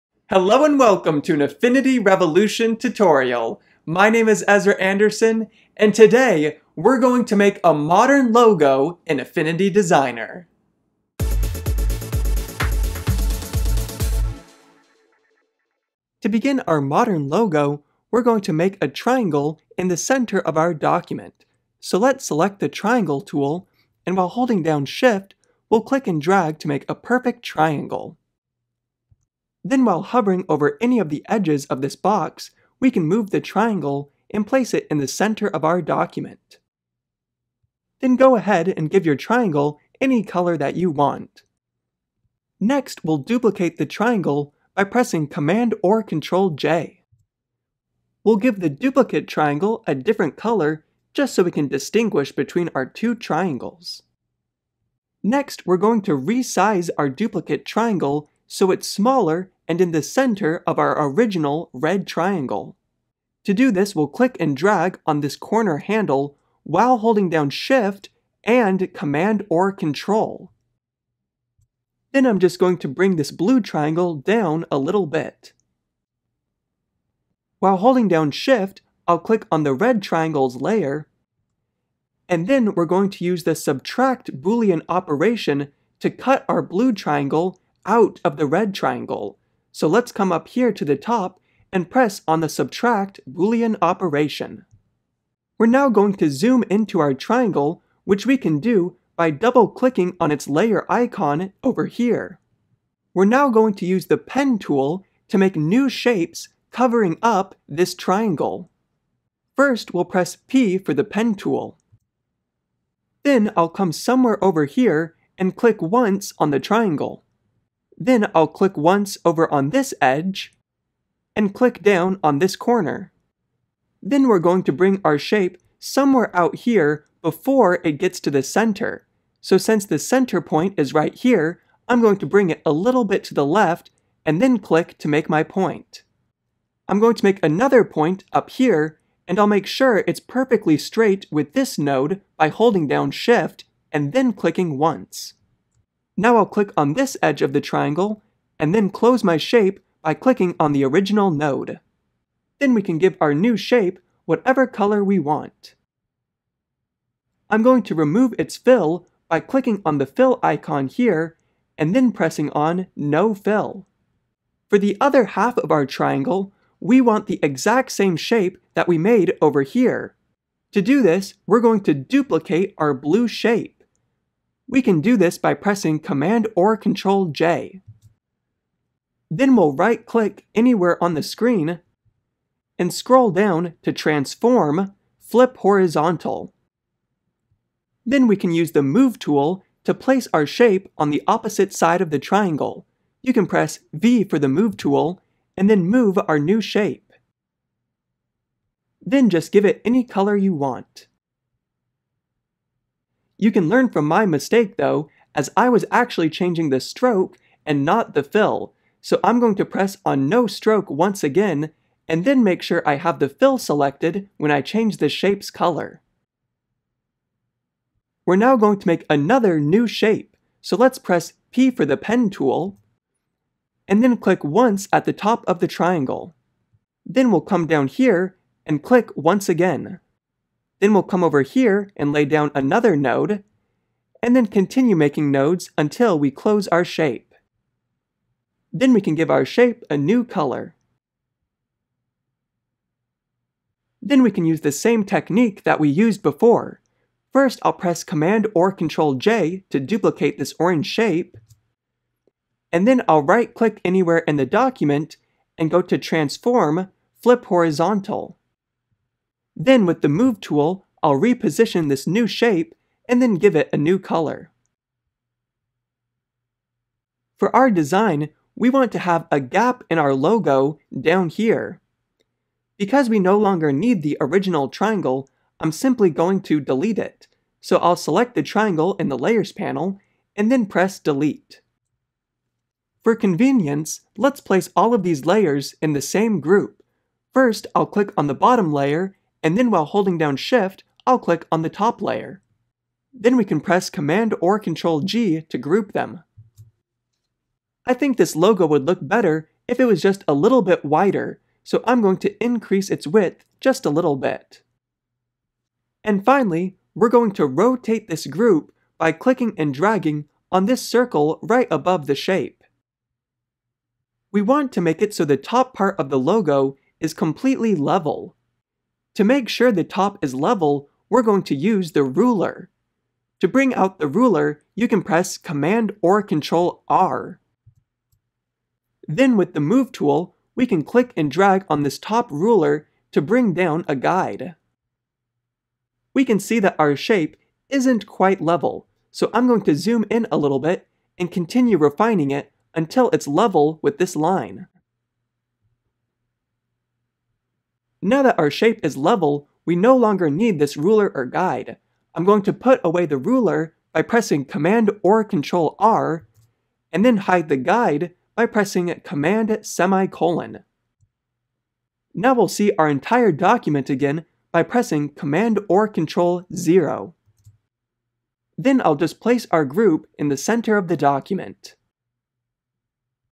Modern Logo - Affinity Designer Tutorial
In today's video, we'll make a modern logo in Affinity Designer. This tutorial is perfect for anyone that wants to make put a hidden arrow in their logo. :-)